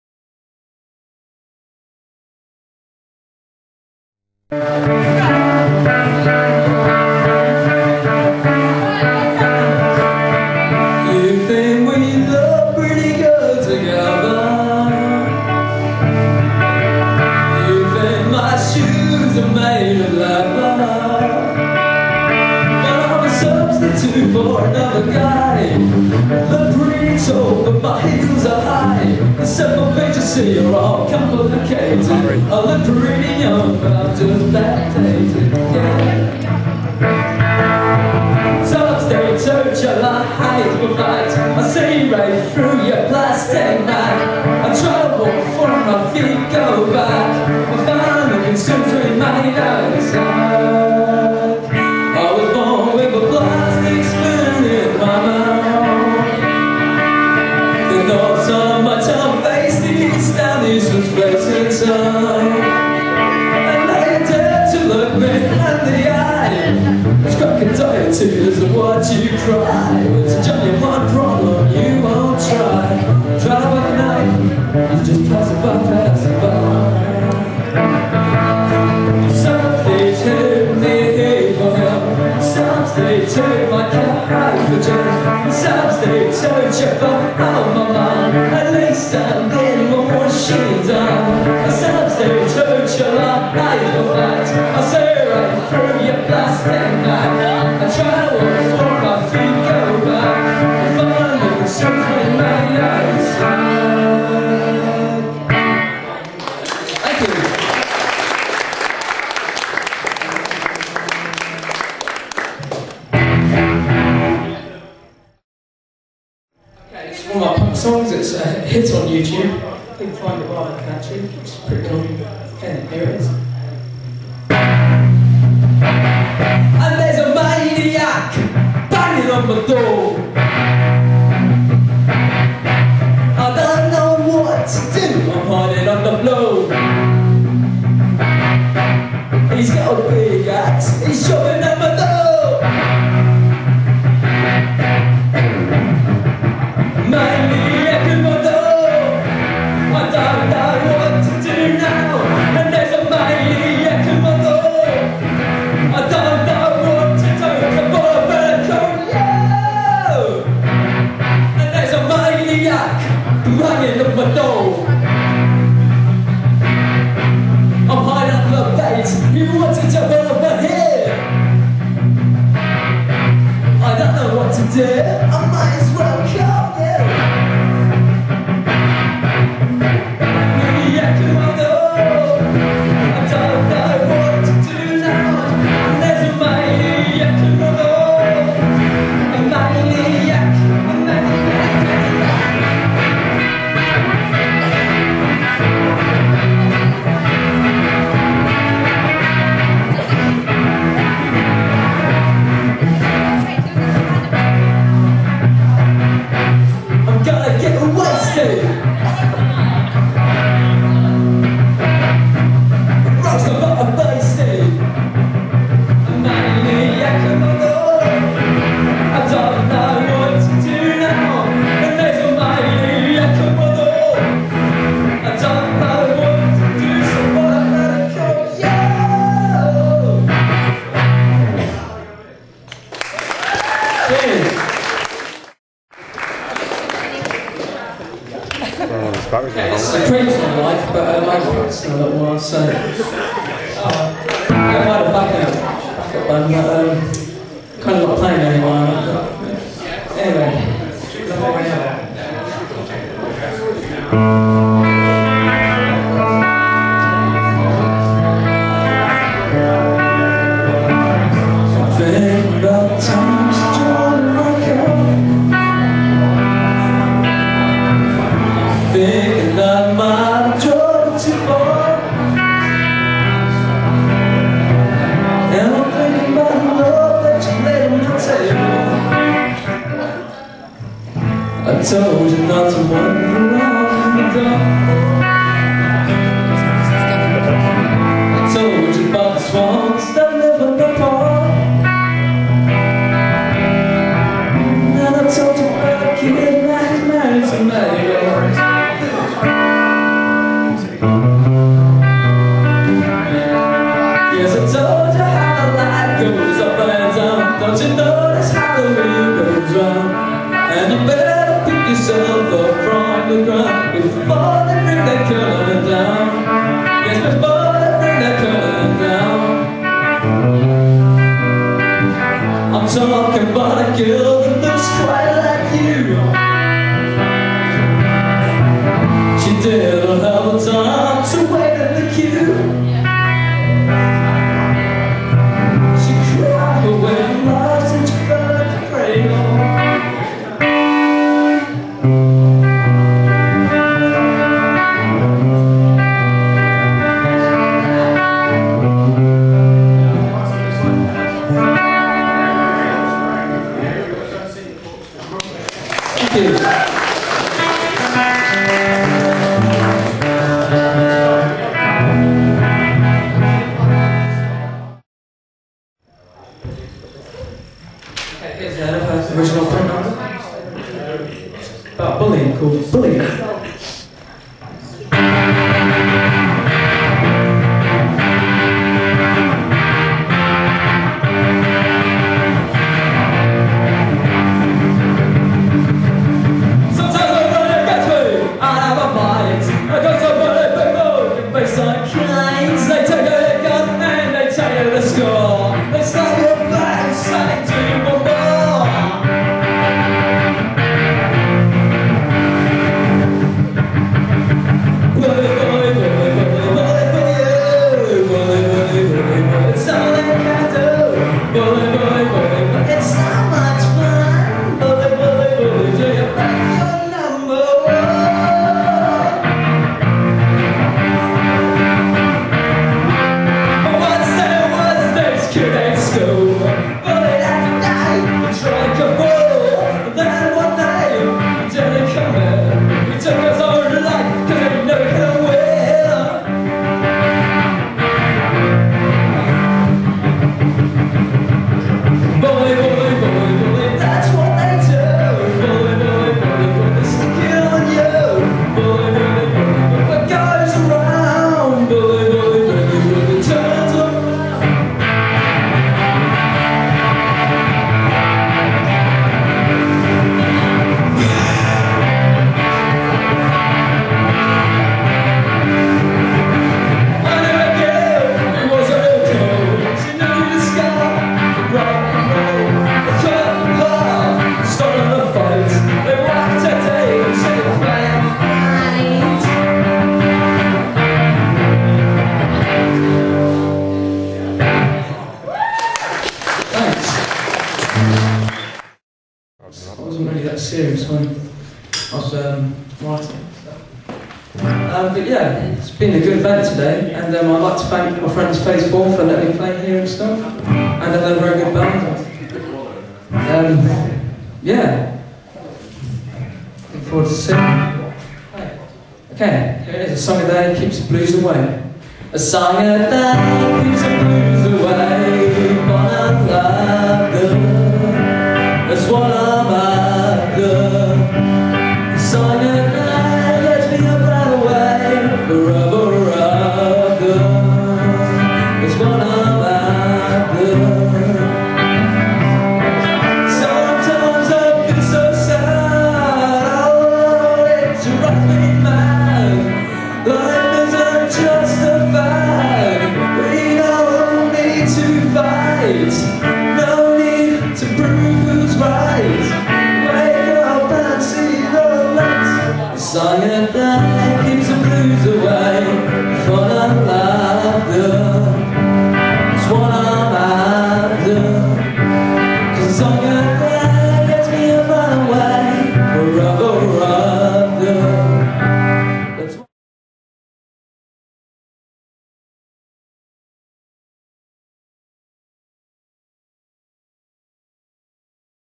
Andy Hughes Live at Thorney Hall
Clips of me playing a live set at a charity gig at Thorney Hall, in Peterborough Cambridgeshire UK.

Note: My singing was very off at this gig, so i'm aware that I cant sing lol